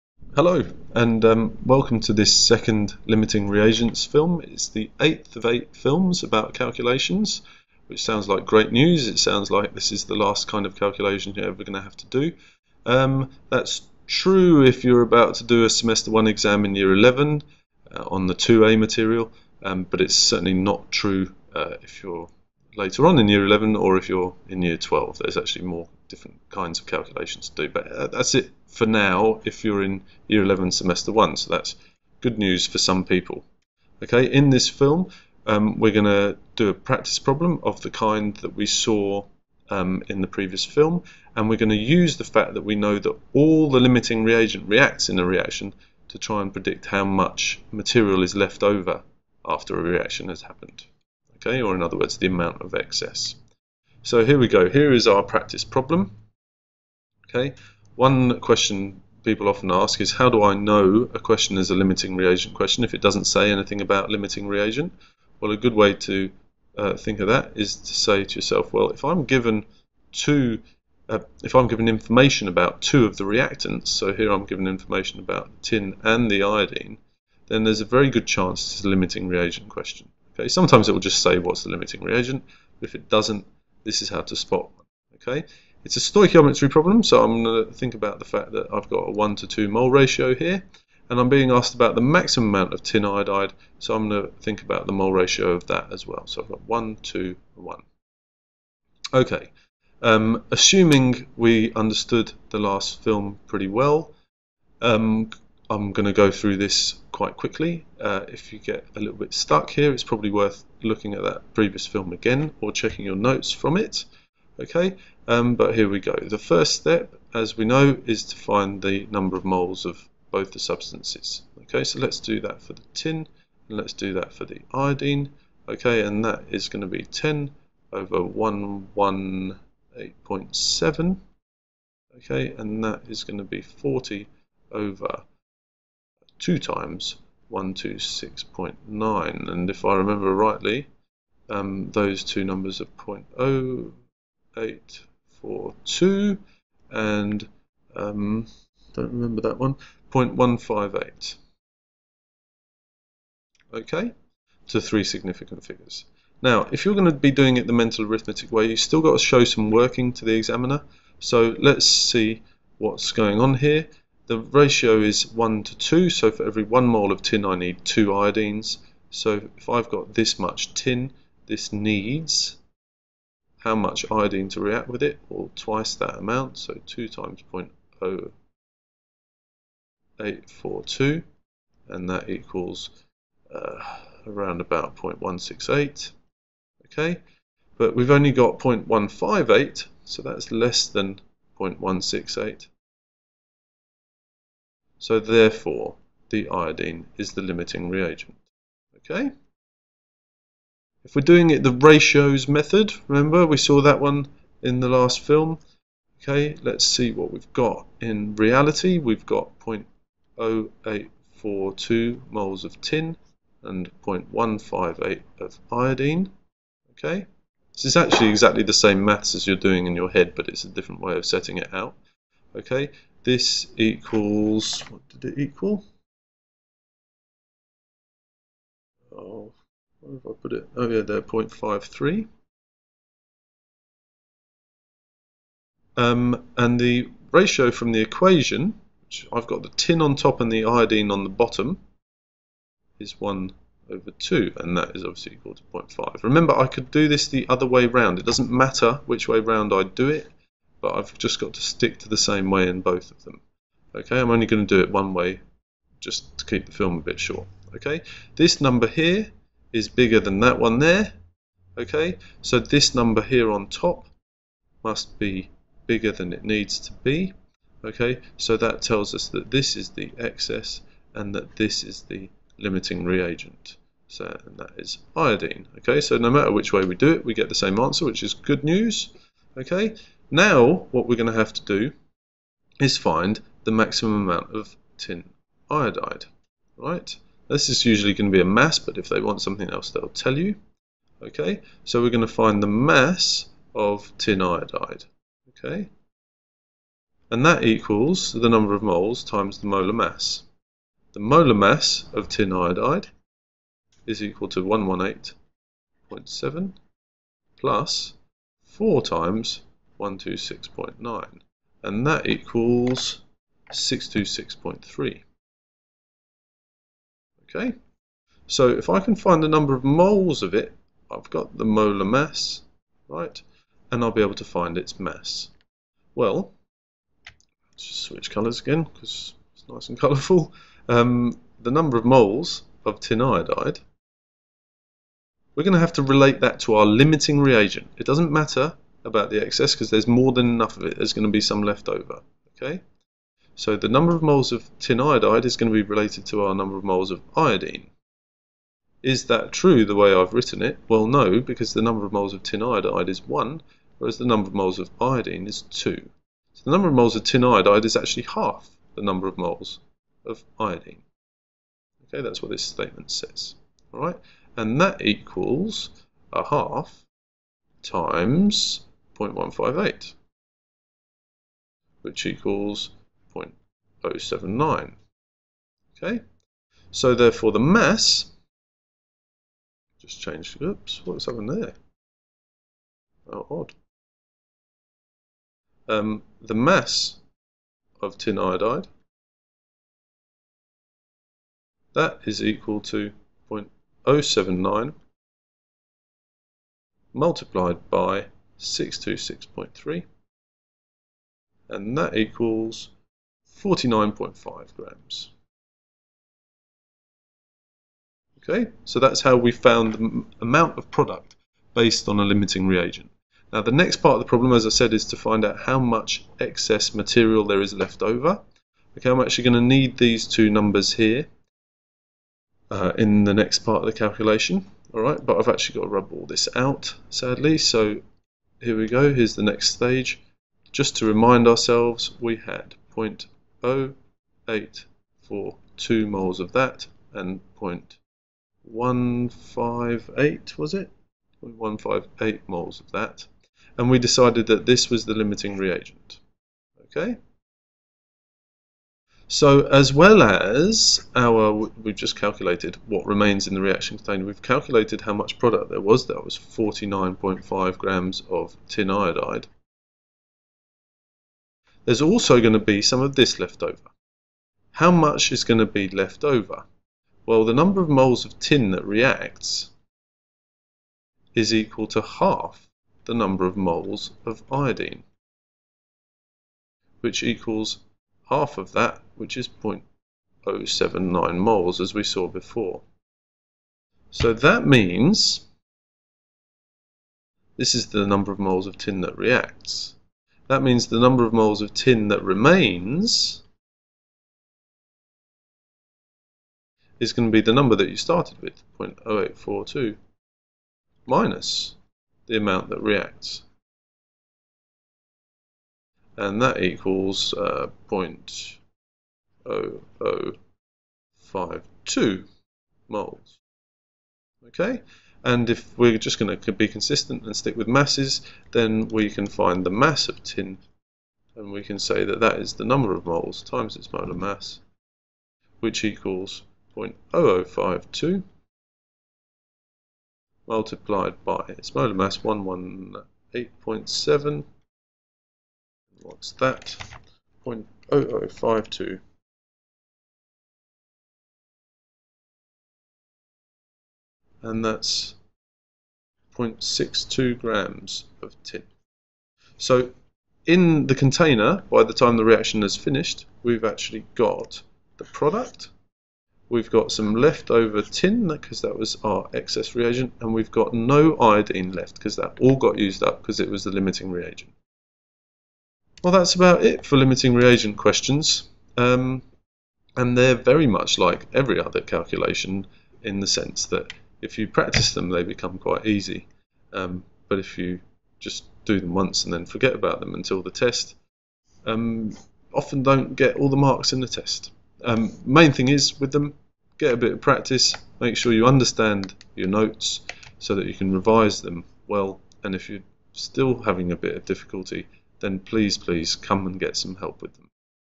8 Limiting Reagents Part 2
8th in a series of six short films covering some of the calculations encountered in the WACE stage 2 chemistry course. Here we look at some actual example calculations involving limiting reagents.